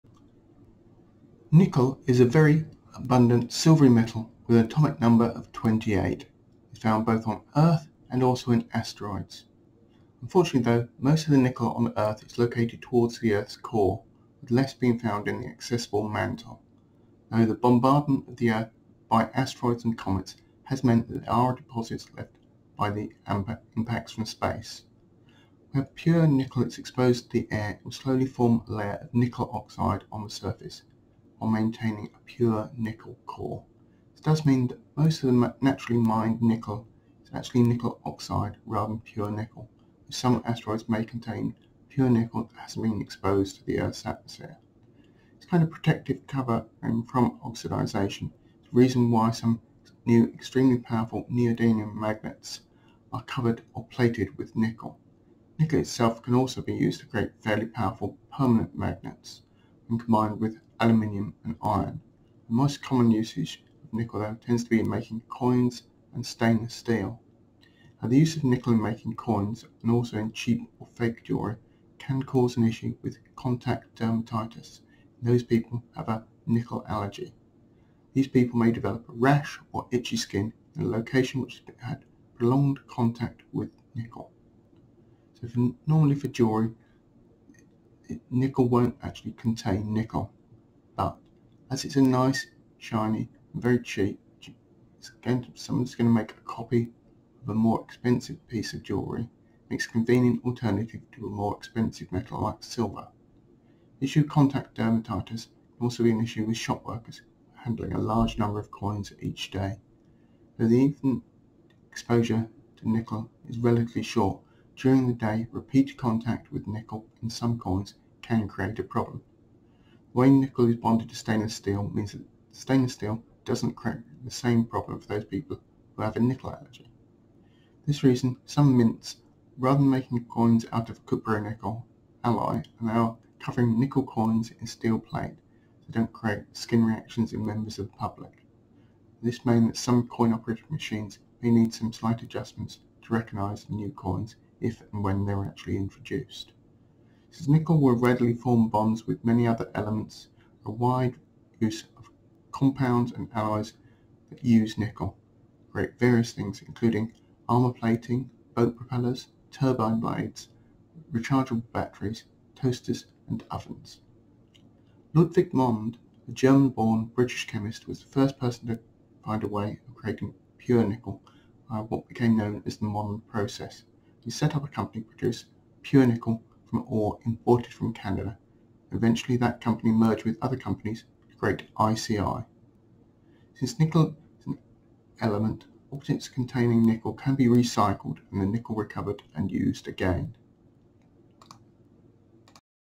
What is nickel and what can it be used for?
Nickel is a silvery metal with an atomic number of 28 and is used in making coins and in stainless steel as well as many other products. Some people are allergic to nickel and can get contact dermatitis from prolonged exposure to nickel. This can be an issue with some fake jewellery and handling coins. Ludwig Mond  came up with the Mond process which was a way of creating pure nickel from mined nickel ore and the company he formed eventually became ICI.